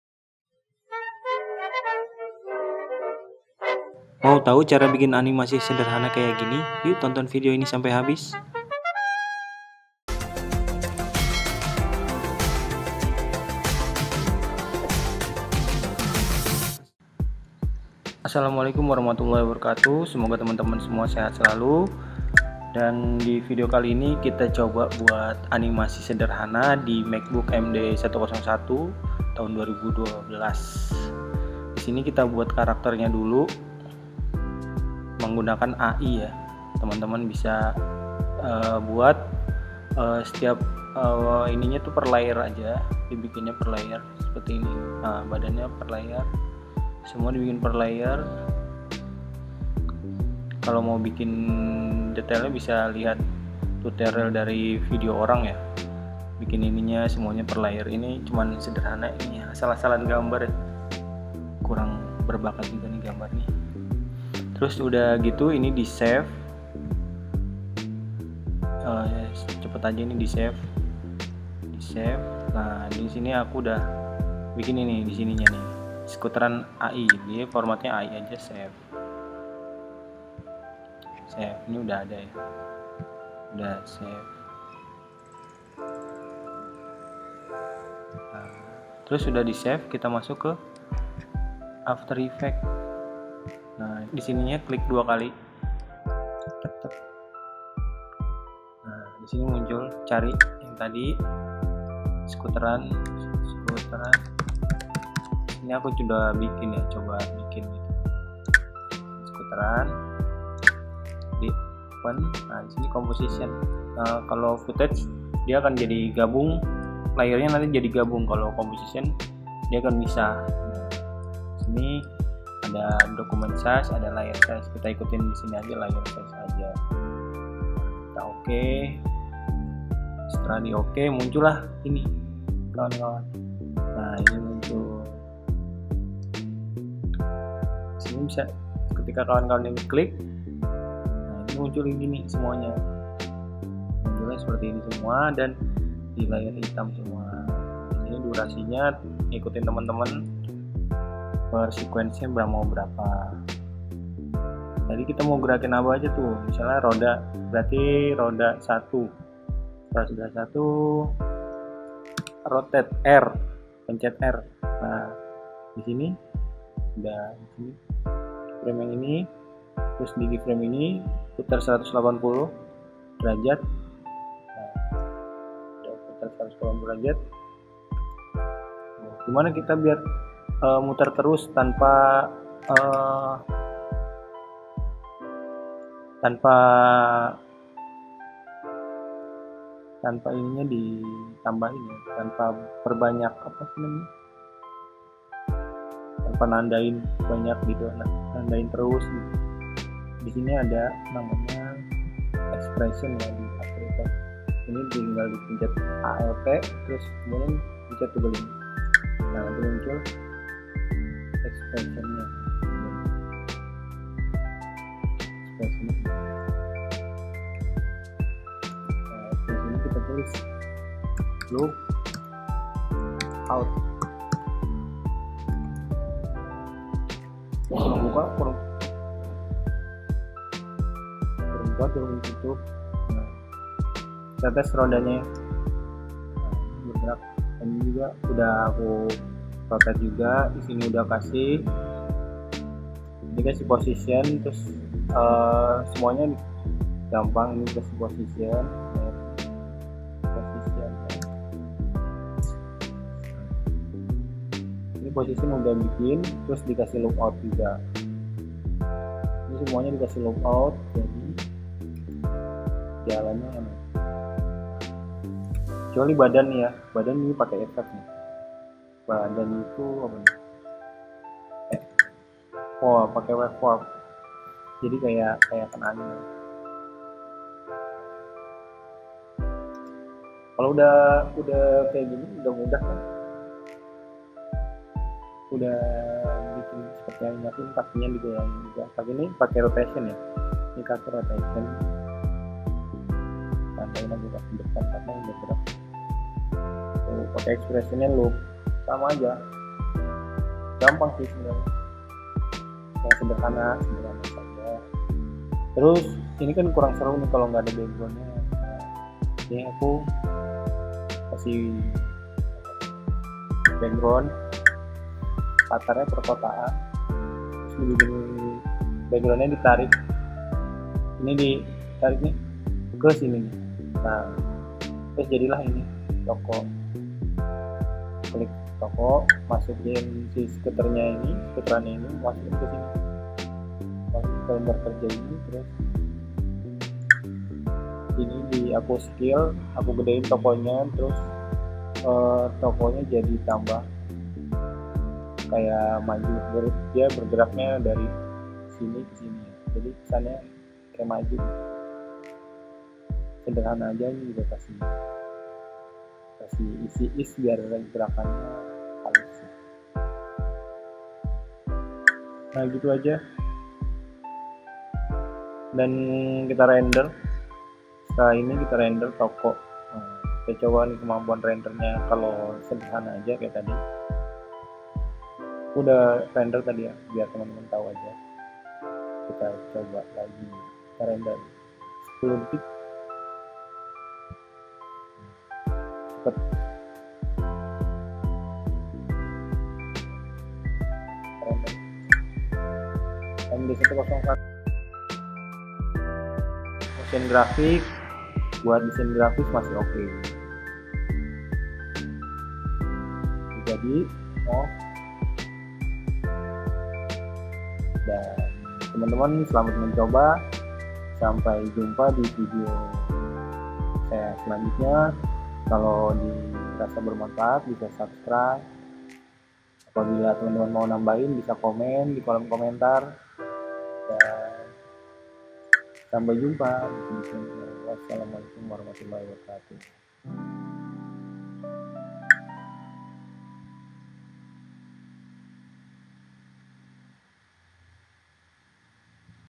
Cara Bikin Animasi sederhana After Effect di Macbook MD 101
ternyata masih oke loh bikin animasi sederhana kayak gini di laptop macpro md 101 pake aplikasi after effect, tonton ya brader sapa tau menginspirasi.